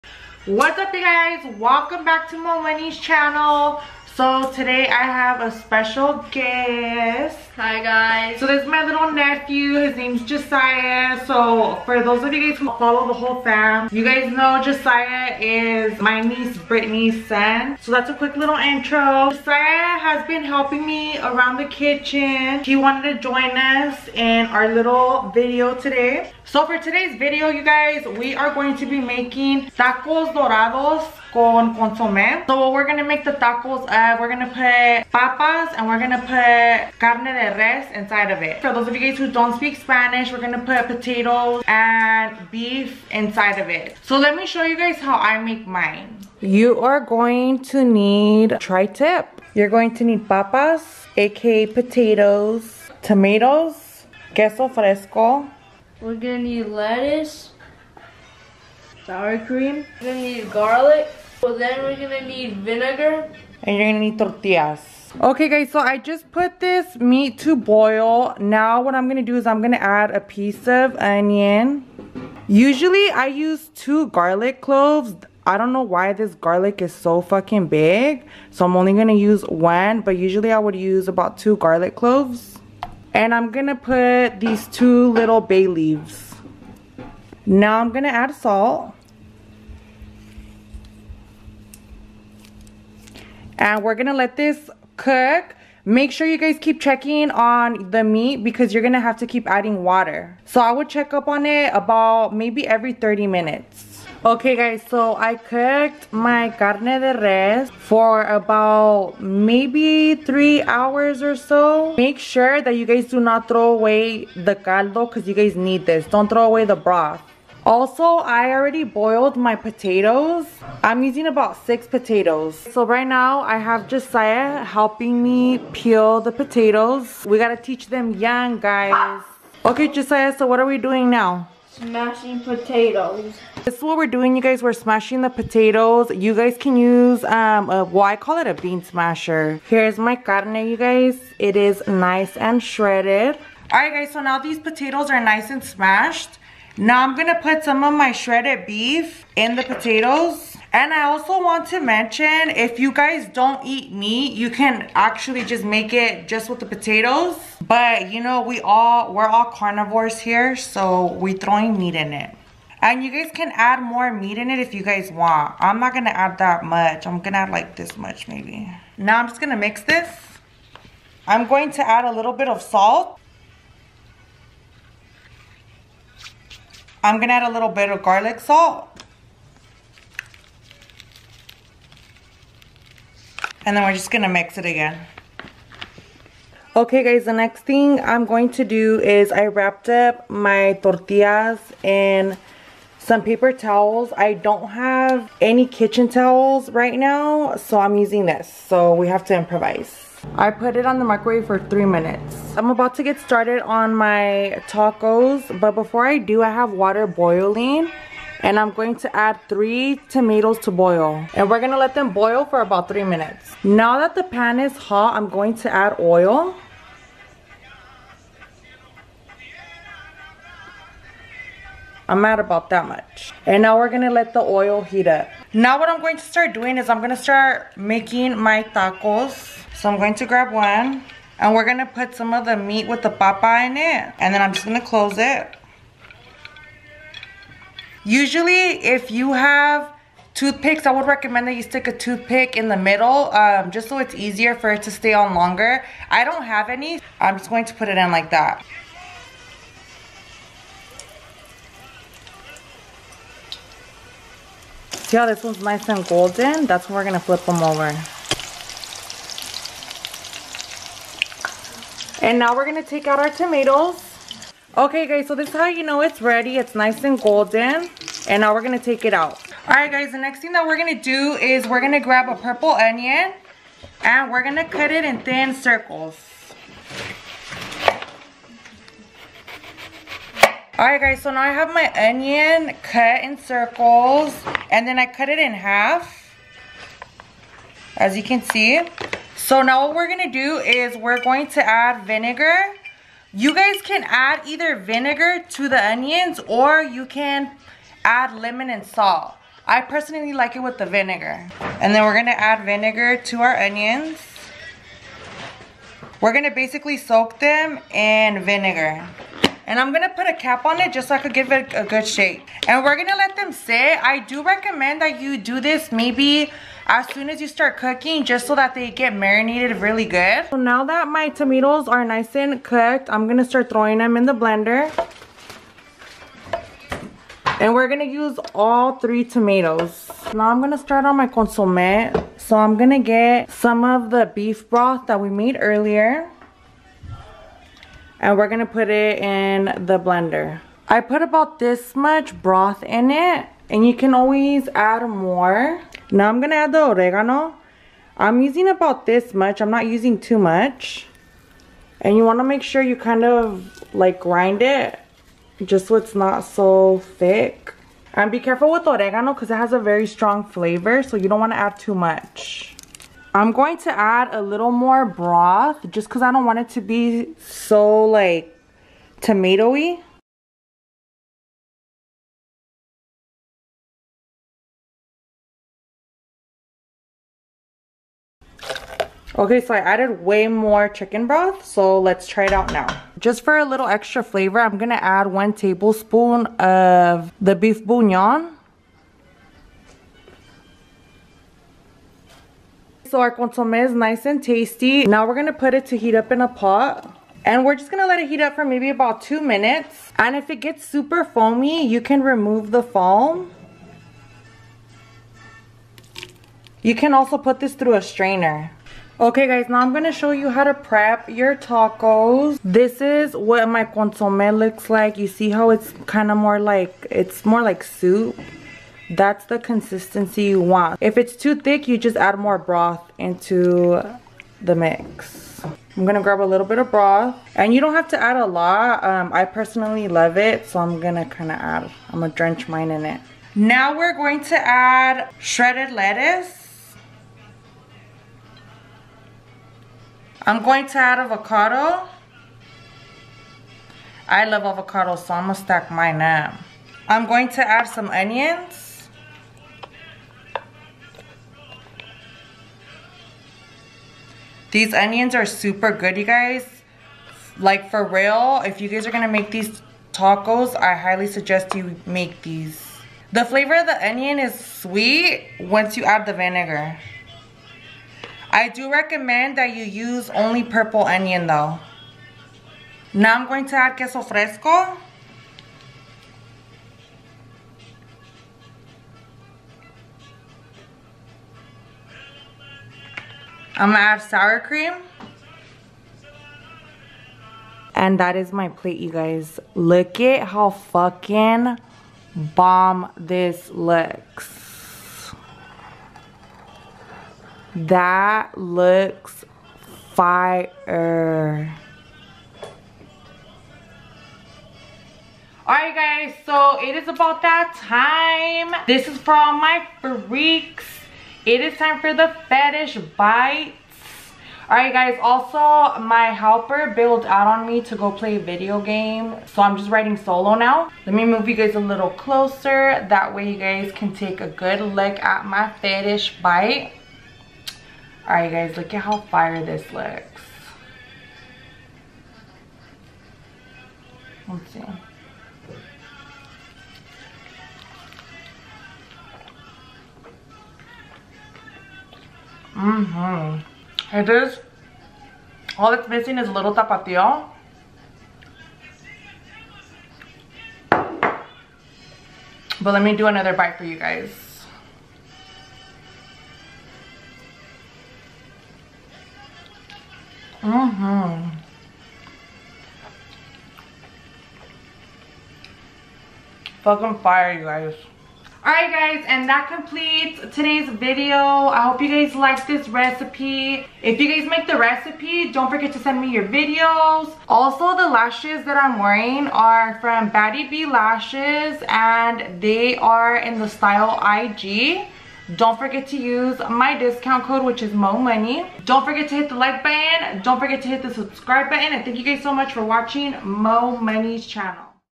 TACOS DORADOS DES RES CON CONSOME
YOU WILL NEED:
TRI TIP
POTATOES
TORTILLAS
LETTUCE
TOMATOES
GARLIC
PURPLE ONION
VINEGAR
SOUR CREAM
QUESO FRESCO
OREGANO

LASHES @BADDIEBLASHES
STYLE: IG